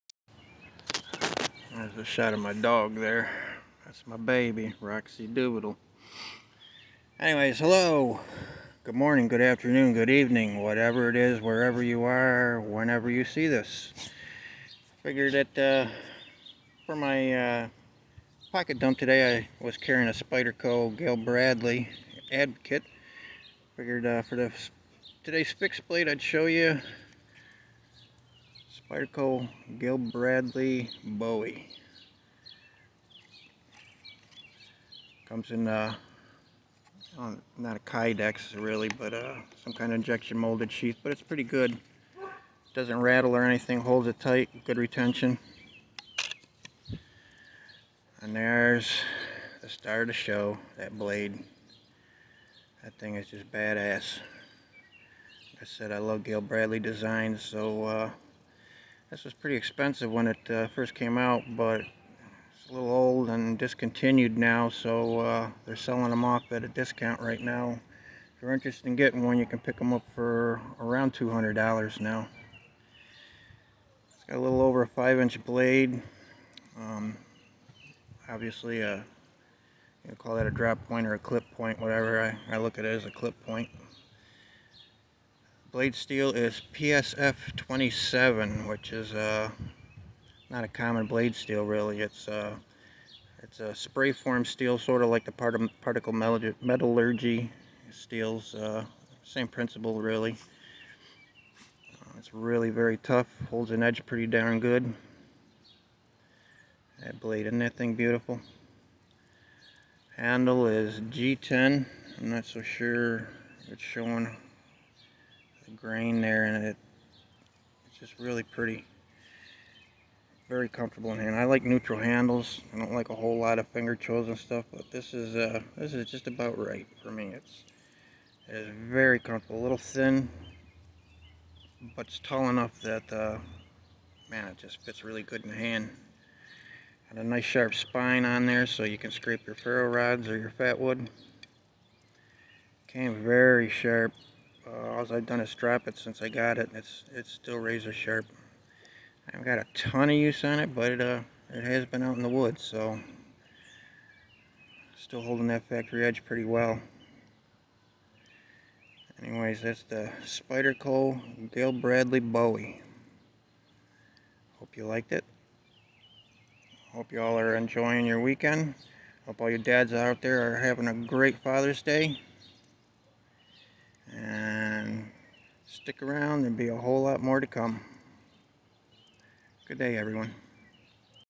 Spyderco Bradley Bowie
A quick look at the Spyderco Bradley Bowie. Hope you like it. If you like knives, stick around. There's plenty more to come, along with plenty of Fatwood Friday Burns!